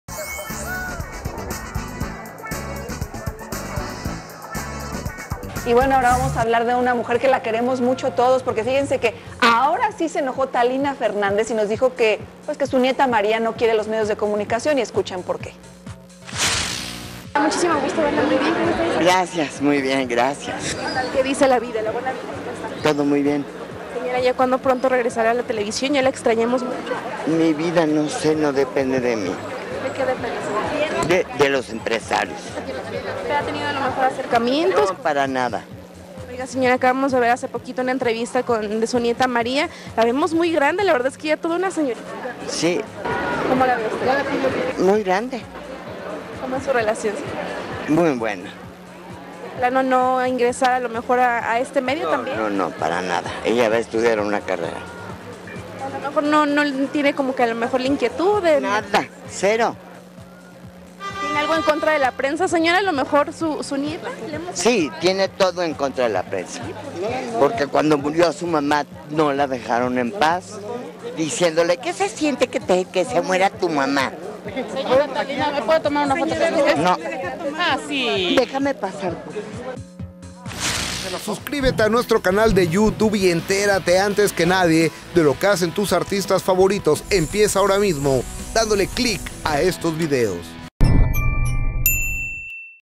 Talina Fernández reveló que su nieta no quiere a la prensa
Talina Fernández reveló que su nieta no quiere a la prensa ante la insistencia sobre la muerte de su madre

18 de junio 2015